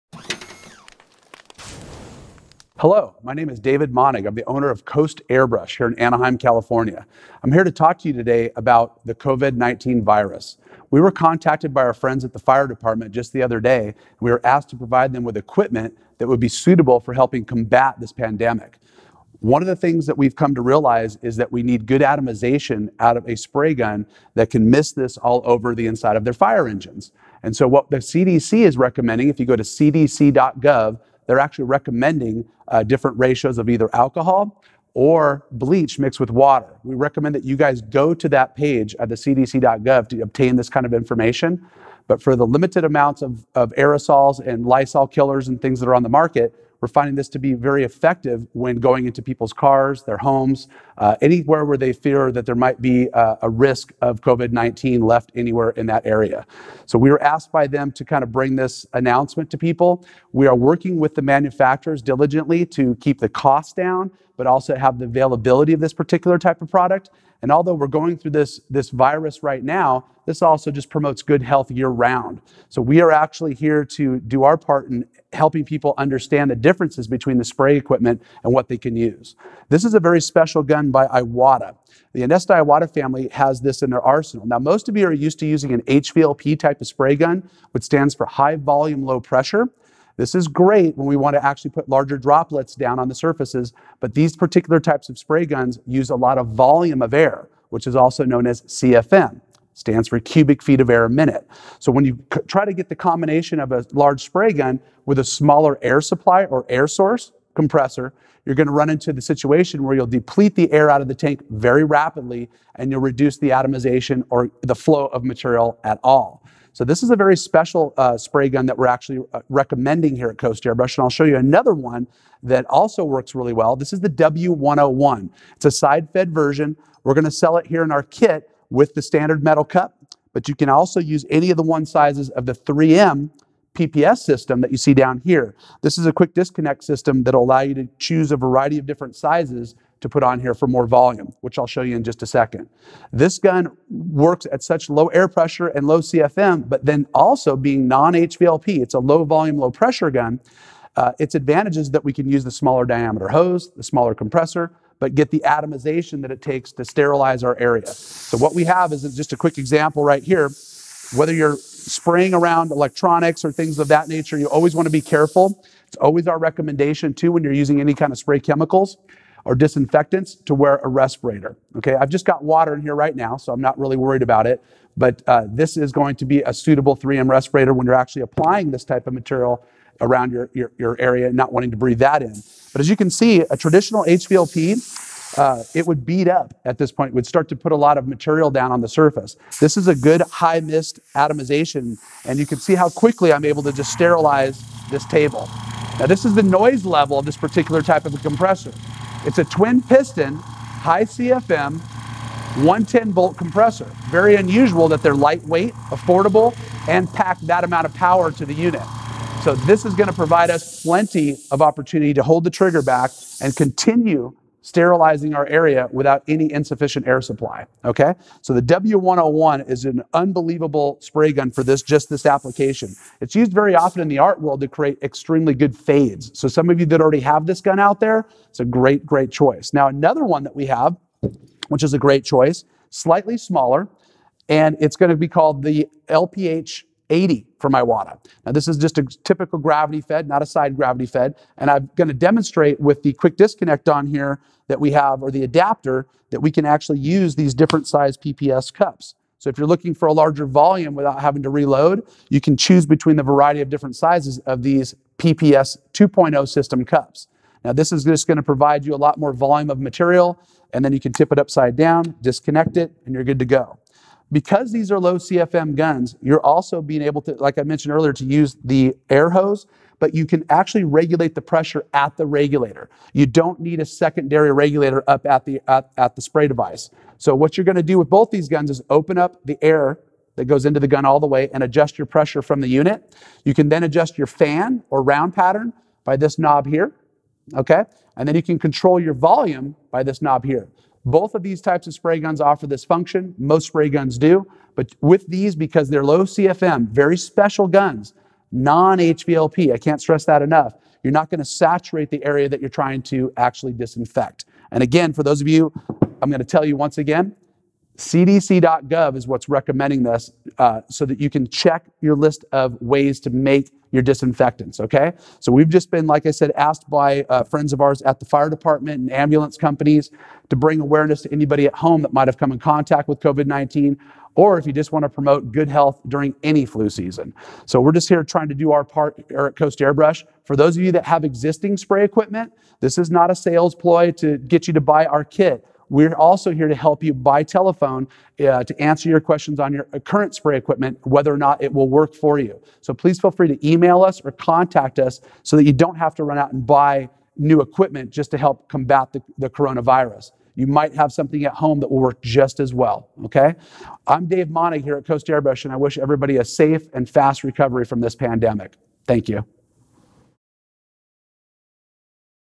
High Efficiency Sanitizing Kit - For COVID-19
VISIT: CDC.GOV FOR CURRENT UP TO DATE CLEANING INFO/SOLUTIONS

With the recent Pandemic of COVID-19, we were asked by professionals in the health/safety division at Fire Departments & Ambulance care for recommendations and a set up to assist in the disinfecting of large areas, including their own rigs and working/living areas. So we set out to create a portable and effective set, including the necessities and proper equipment matchings to provide a HIGH-ATOMIZING, Low-CFM operation, that is capable of distributing product in a scale not able by aerosol disinfectants or wipes. This set has all of the appropriate connections and accessories to plug and get to work! All products manufactured in USA, Japan & Mexico.
Visit the CDC website above for up to date information on homemade disinfecting solutions or chemical solutions manufactured for such uses.